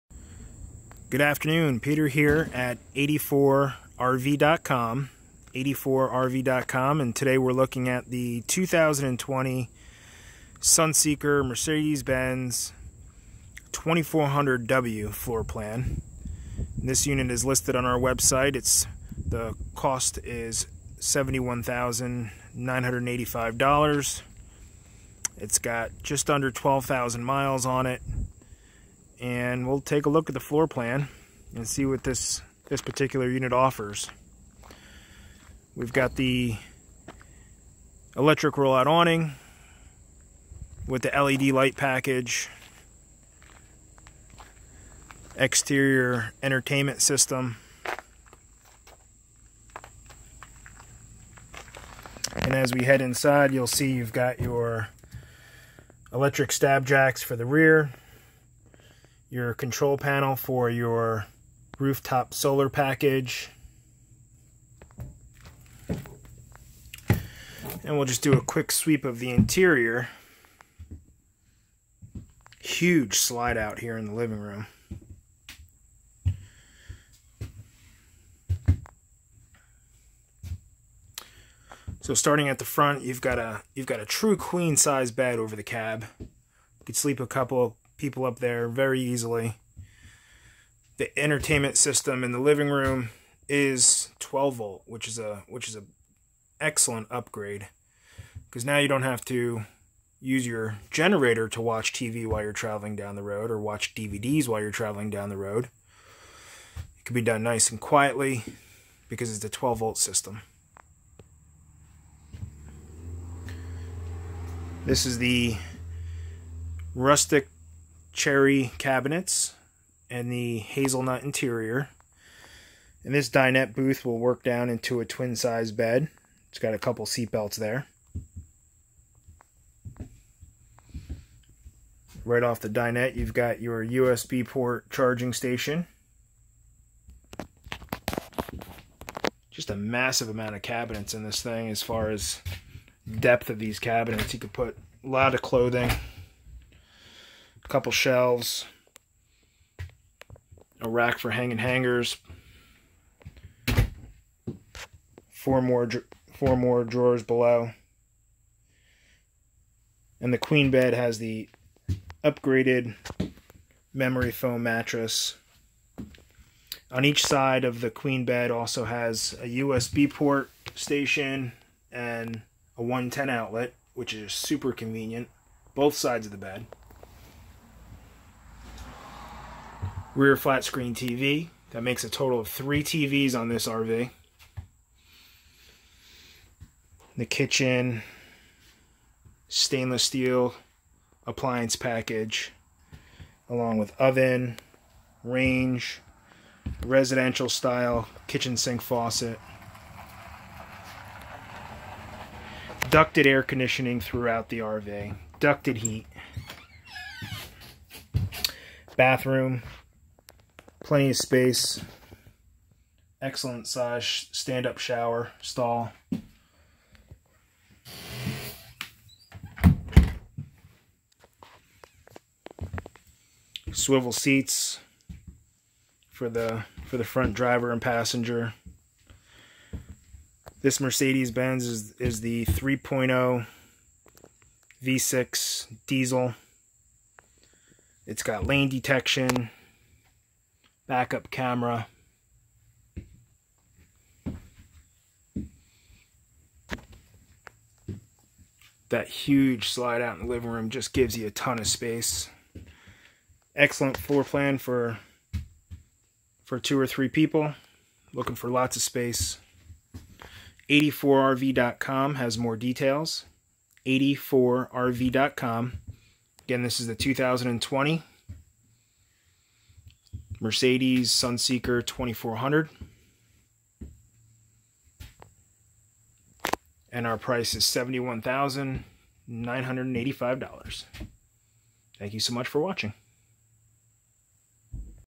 Used 2020 Forest River Sunseeker Mercedes 2400w -- SOLD
2020 Sunseeker 2400w Mercedes-- SOLD

Sleeps 5 , Queen Bed with Upgraded Mattress, Dinette/Bed, Sofa/bed, Queen Bunk Over The Cab, Full Kitchen with Stove/Oven, Microwave, Front & Rear Flat Screens TV, 4000 watt Generator, Electric Power Awning, Electric stab jacks, with Integrated LED Lighting, Back Up Camera, Running Boards, Heated Remote Exterior mirrors, molded front fiberglass exterior cap.  leather front seats.   Roof Solar package! Stainless Steel Appliances  USB ports Throughout Includes Spare Tire. Side View Cameras Outside TV Entertainment center Oven 3.0l V6 Bluetec Diesel Engine Front TV & DVD systems are 12 volt; that means NO GENERATOR NEEDED to watch TV while camping or in transit! Trailer Hitch with 7-pin Round Connector, Residential Vinyl Flooring Throughout.   NO DOC/PREP FEES Includes full orientation of how to use the RV.